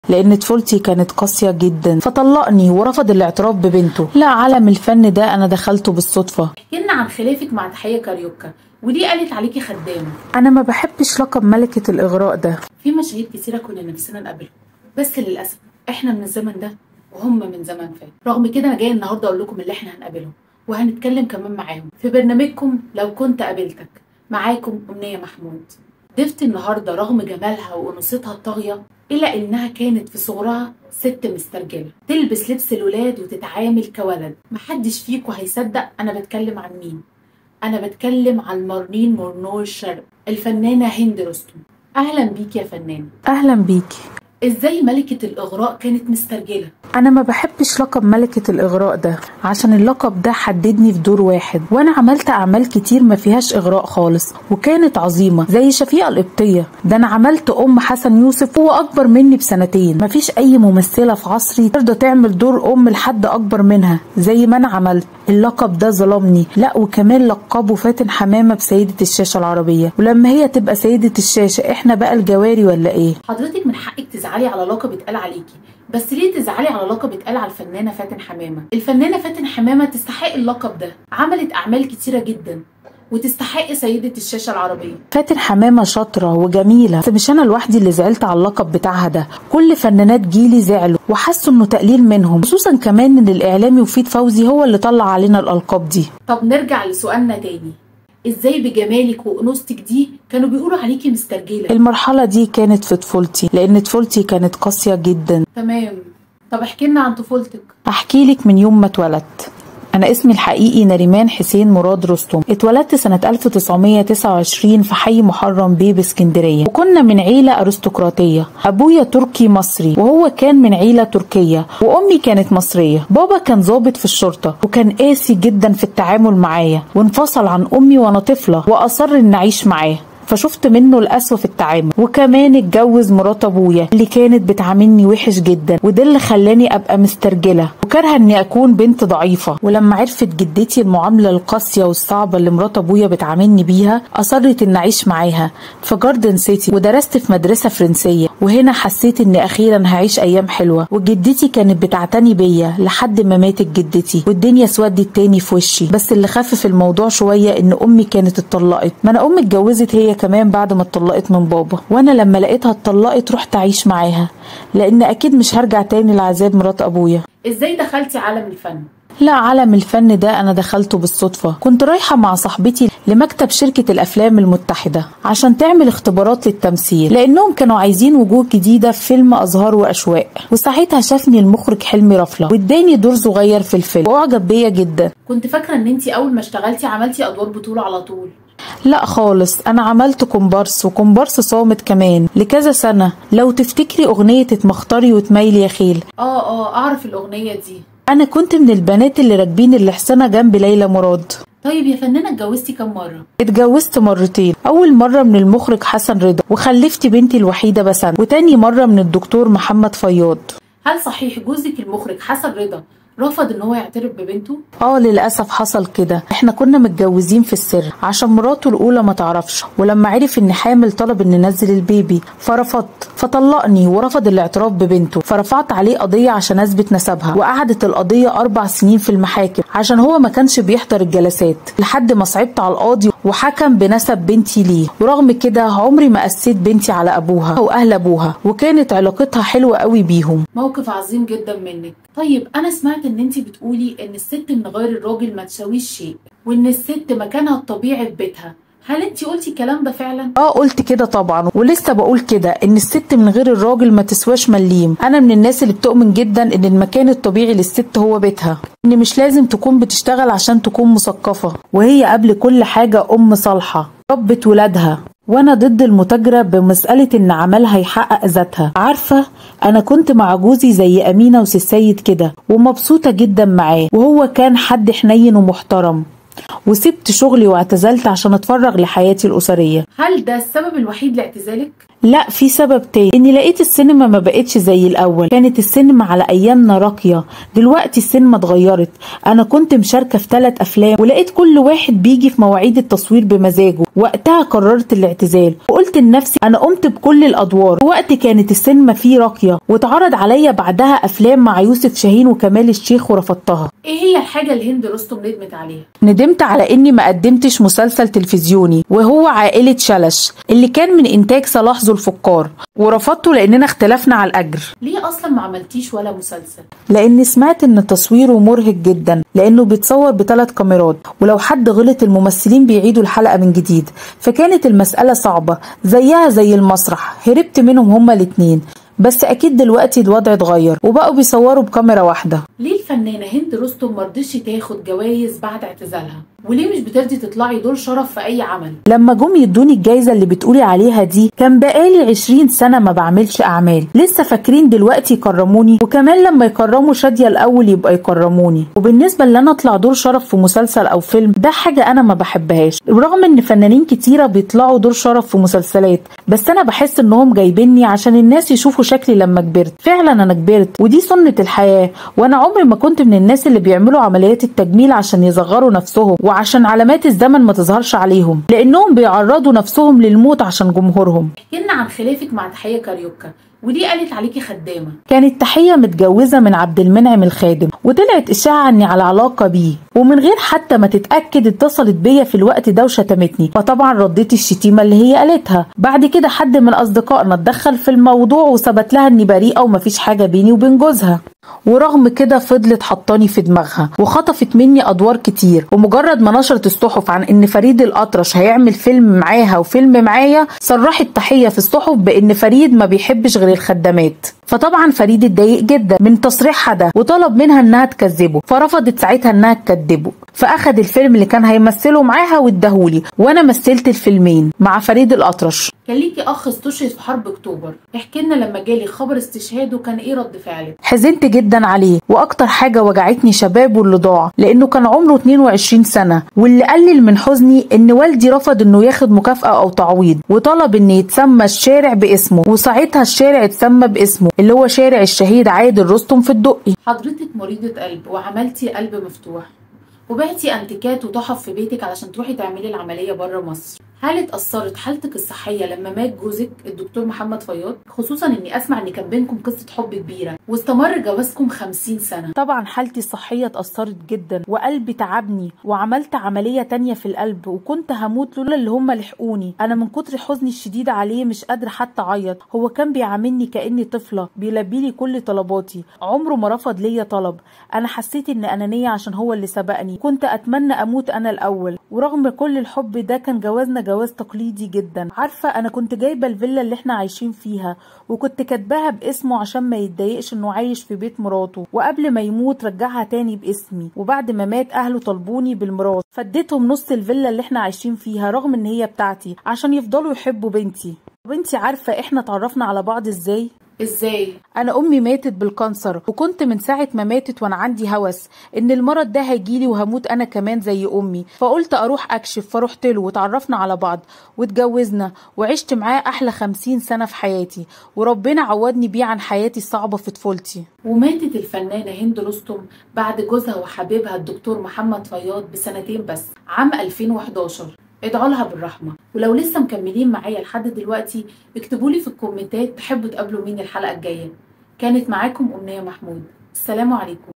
بالذكاء الاصطناعي هند رستم تحكى عن قضية " نسب ابنتها " و خلافها الشهير مع تحية حين وصفتها بالخادمة
النهاردة هحكى لكم عن هند رستم و قصة حياتها كاملة ف برنامج "لو كنت قابلتك"